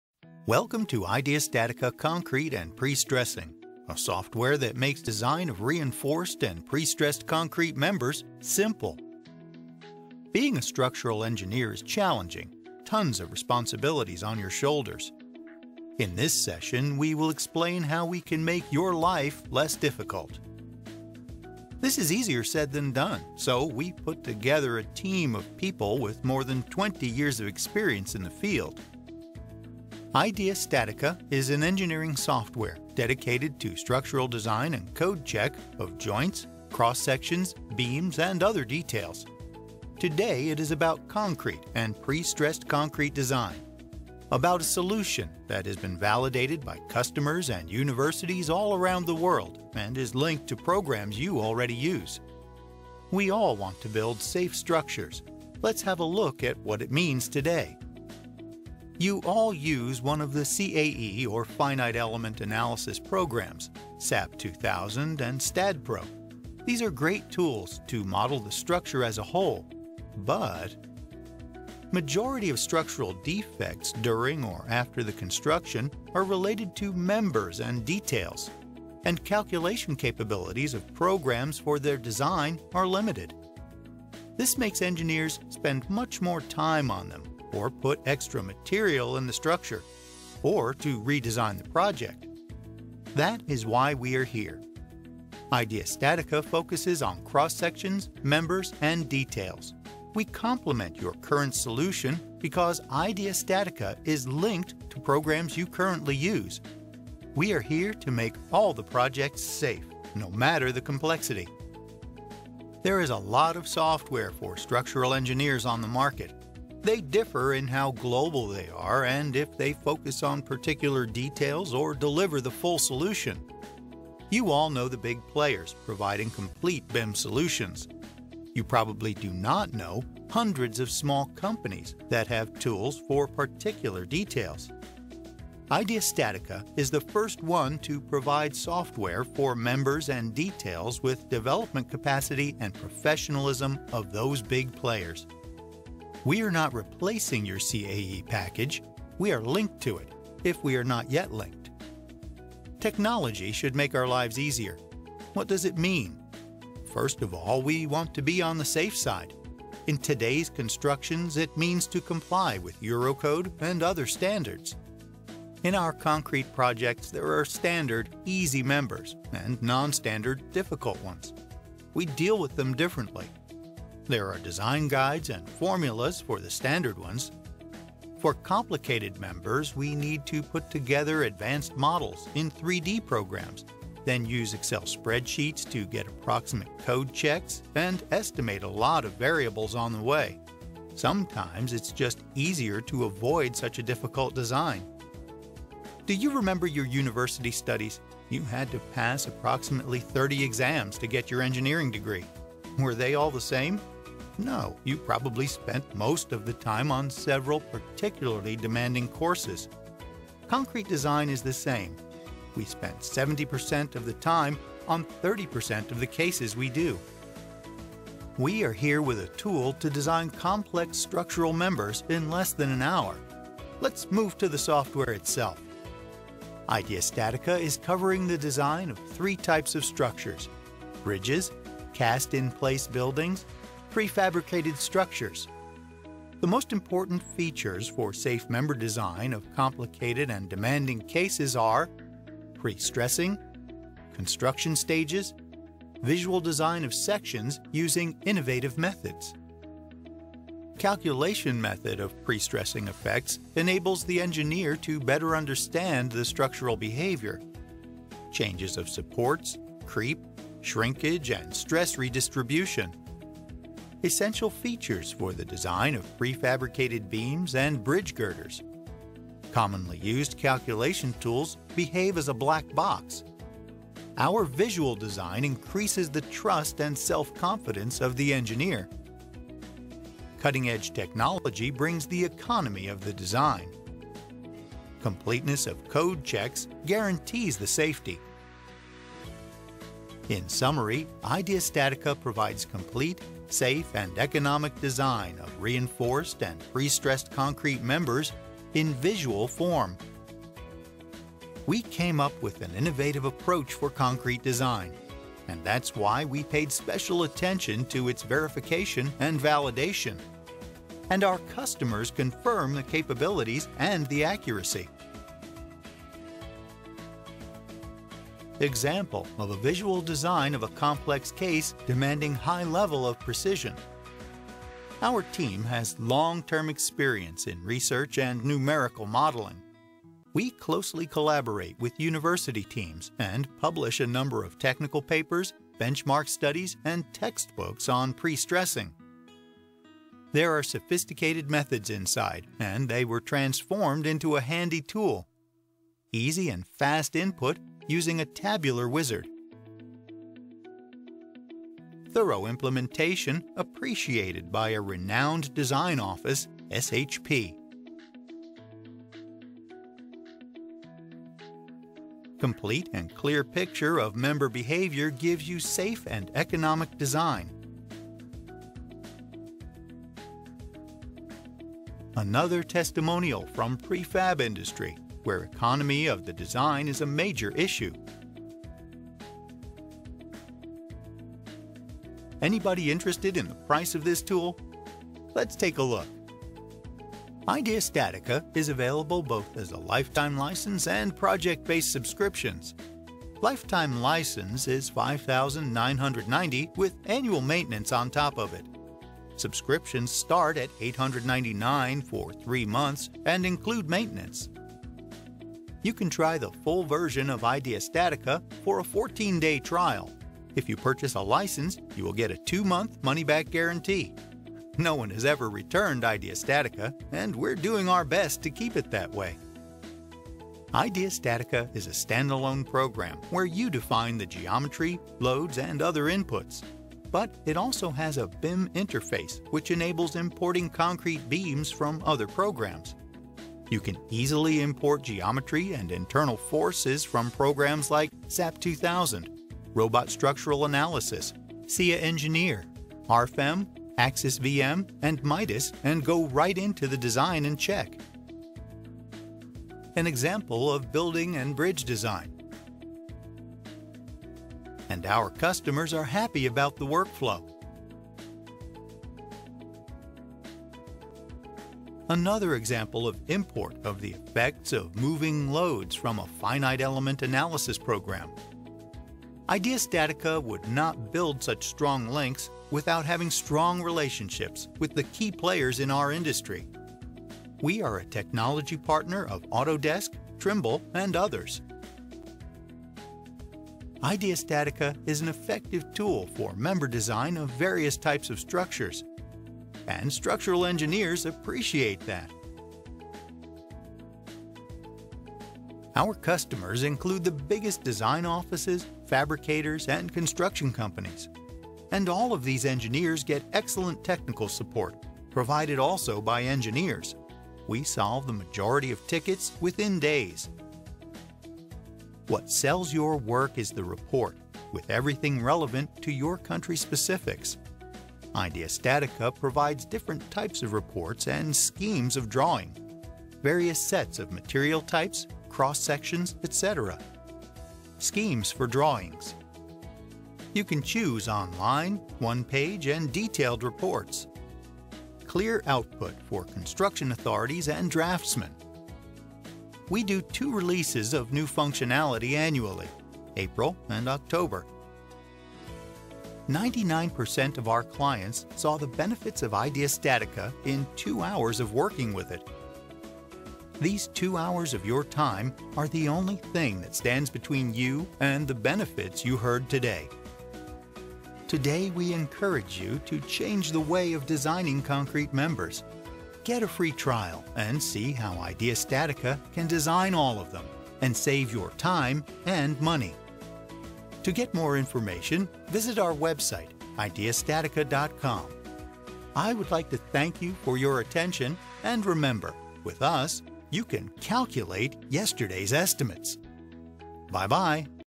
Why IDEA StatiCa Concrete?
EC/SIA design of members, cross-sections and details of reinforced and prestressed concrete structures.

IDEA StatiCa Concrete & Prestressing enables an effective design of all reinforced concrete, prestressed and composite members, cross-sections and details.
Structural checks comply both Ultimate and Serviceability limit state requirements, including deflection check considering cracking. Structural analysis of precast members and bridges is performed as time-dependent analysis with respect to construction stages.